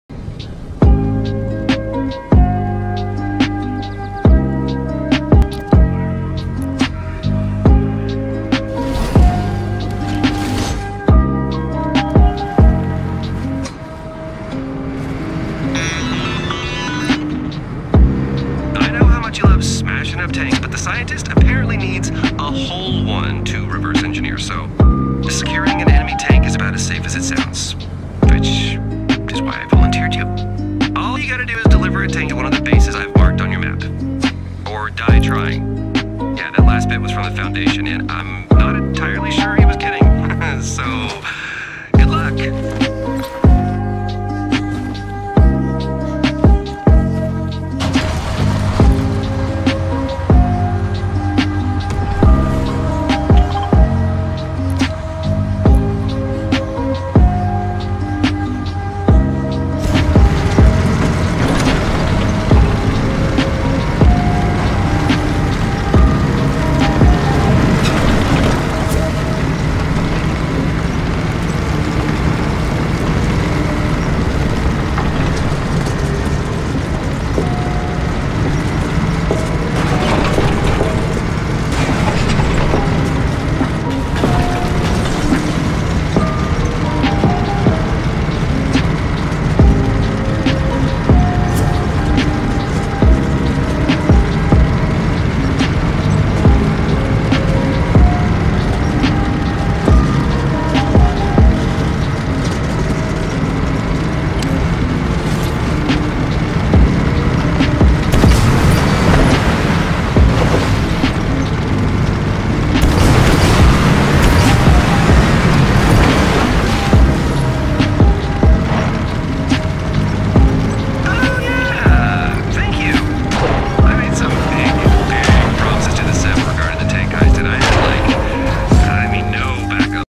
I'm sending you on an Extremely well armored joyride jones | Deliver a tank to a Seven Outpost
Why'd the banana cross the bridge? To learn how to drive
Refuel a vehicle
Establish Device Uplink  Near the central gas station
A car's gotta have gas
Stay on the Road !
Cross Behemoth Bridge in a vehicle
XP Glitch